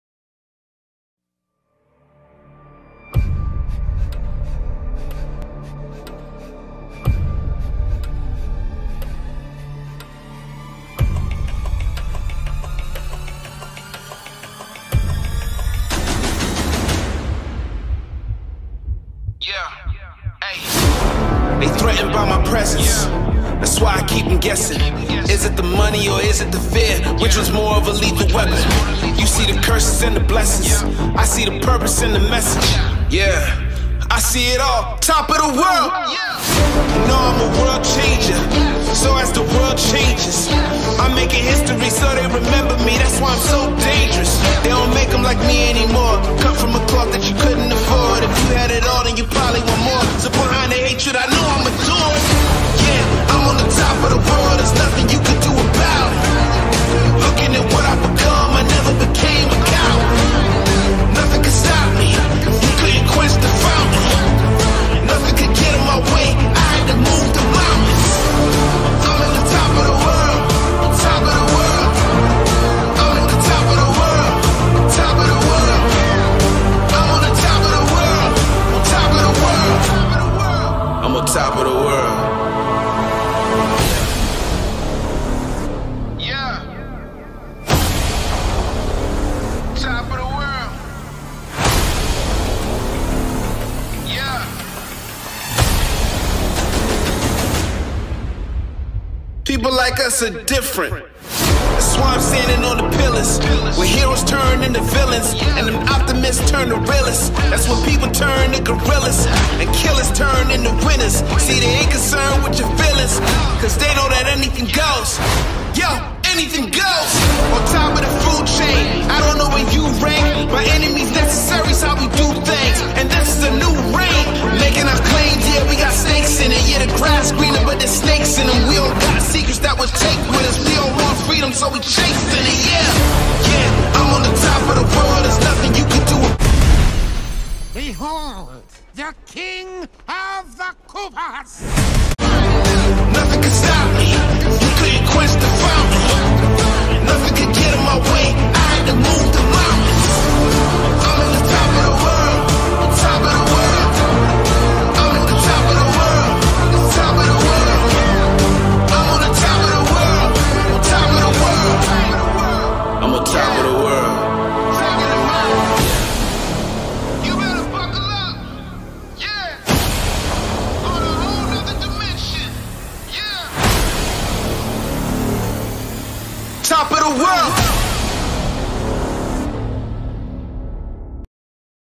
Seven Headquarters Update ONCE AGAIN
Including Reality Trees and Rave Cave props, And props I don’t know where they belong!